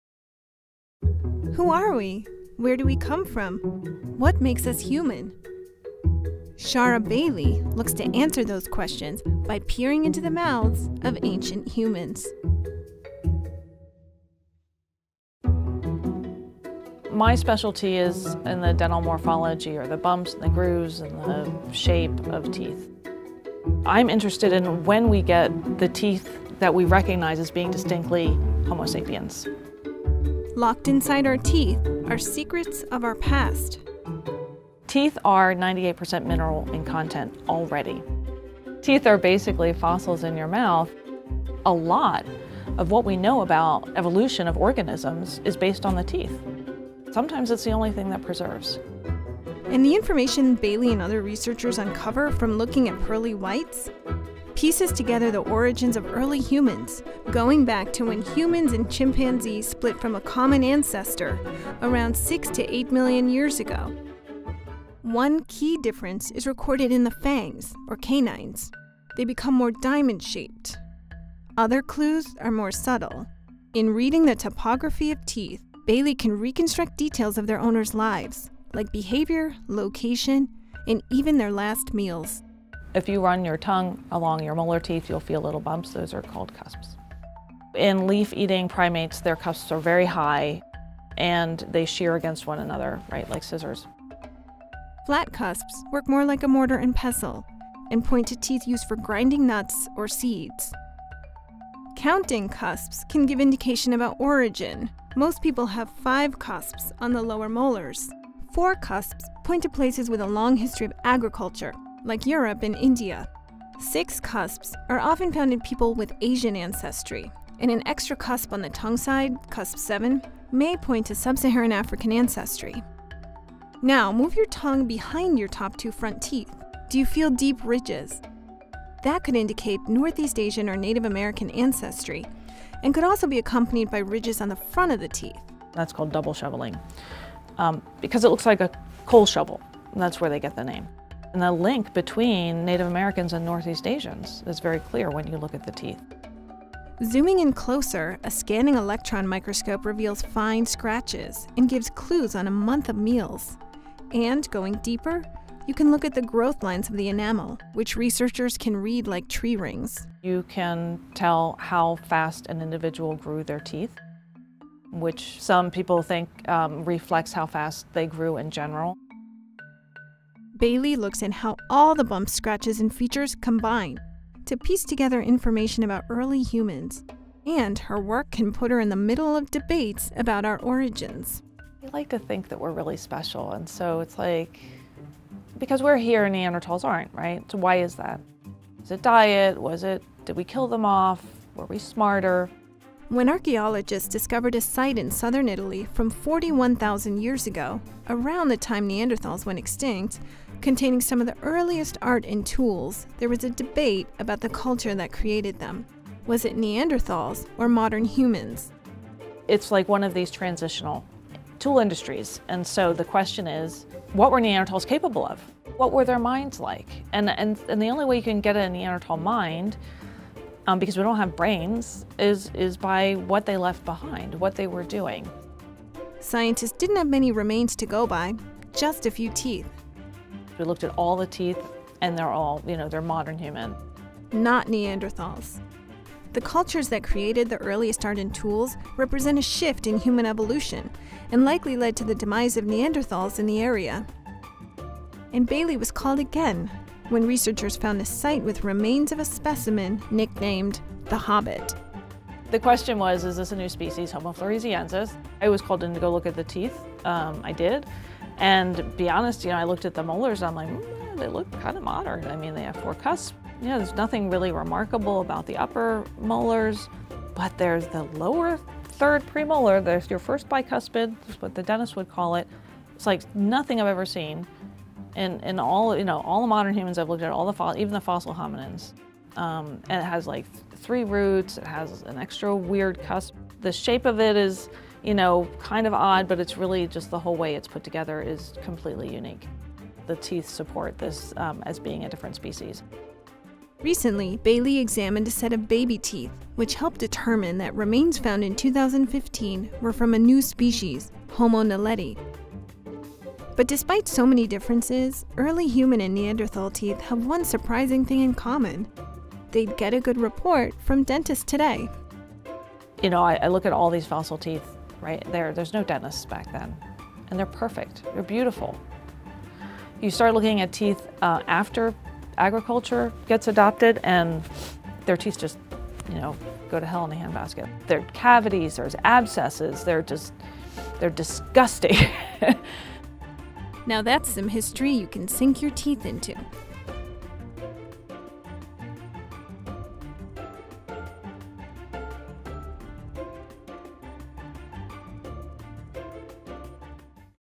The Wisdom of Teeth
Ancient human teeth can tell us a lot. Hidden inside each set are clues about their owner's behavior and ancestry plus hints about what really made up the paleo diet. Shara Bailey, associate professor of anthropology at New York University, reads the topography of teeth to better understand the origins and lineages of humans. You can even test your own teeth to see if you have the same bumps and grooves as your ancestors.

Produced and Narrated by Emily V. Driscoll
Filmed by Jeff Nash

Additional Video by POND5
Images by

Cicero Moraes (Arc-Team) et alii, Daniele Panetta, CNR Institute Physiology
Margherita Mussi, Patrizia Gioia, Fabio Negrino, Thilo Parg
Rosino, Wellcome Images

Thanks to Cara Biega and James Devitt